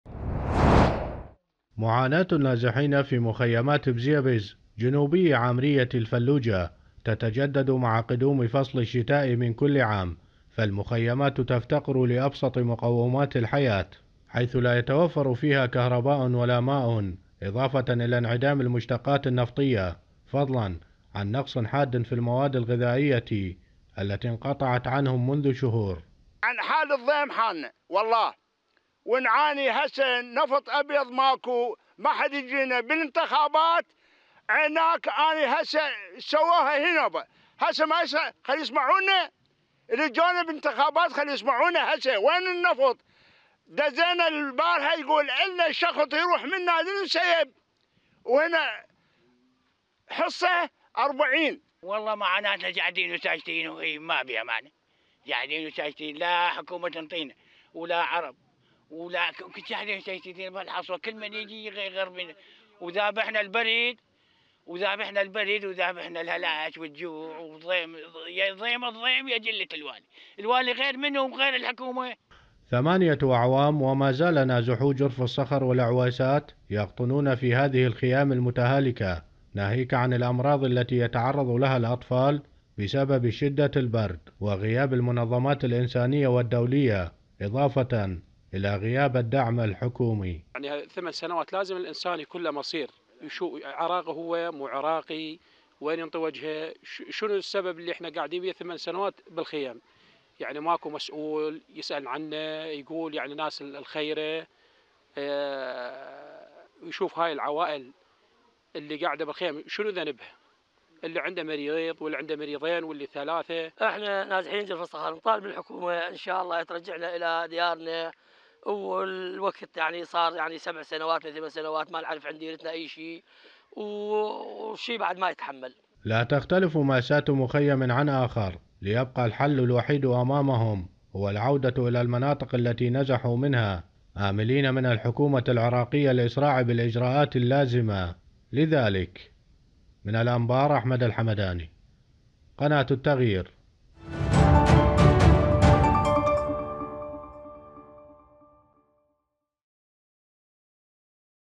هل تستطيع العيش هنا ؟! | تقرير
نازحو مخيمات بزيبز في محافظة الأنبار.. معاناة إنسانية يفاقمها البرد القارص شتاءً والحر القاتل صيفا.. دون أن يلتفت إليهم أحد من المسؤولين أو المنظمات الإنسانية على حد قول النازحين...

تقرير - أحمد الحمداني
موقع قناة التغيير الفضائية الالكتروني